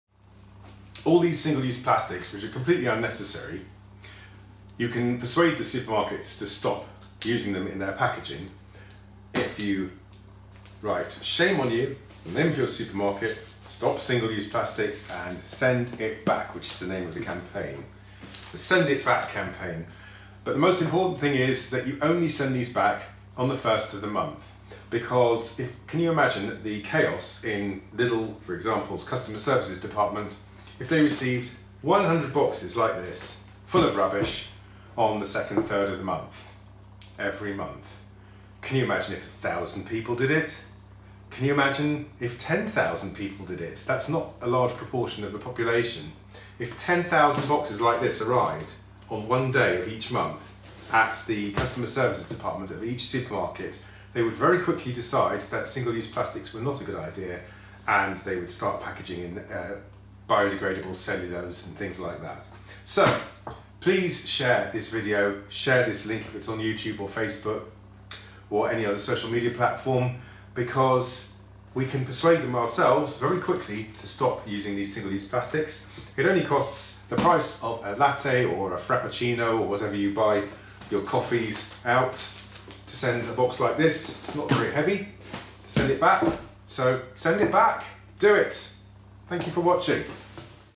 Stop single use plastics... SEND IT BACK!
How to stop the supermarkets from packaging everything in single use plastics... inundate them with their own rubbish by sending it back on the first of every month!